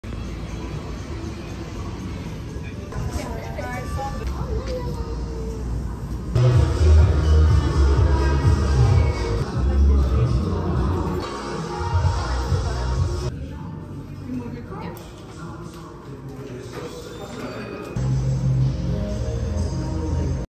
BTS V ARTSPACE: TYPE 1 by Kim Taehyung Pop Up Exhibit in Santa Monica
Decided to swing by last minute today since it was near my work. No photos/videos were allowed of the exhibit but the photos...girllll, they felt so intimate and domestic. They didnt have the tulip keychain I wanted so I'll try to get it next time when I come with my friend. I was also tempted by the cloth poster thing and the magnet and photobooth keychain gaah. There's also a mixsoon event happening right next door and they are giving freebies if you join their class so make some time if you can to get those toners!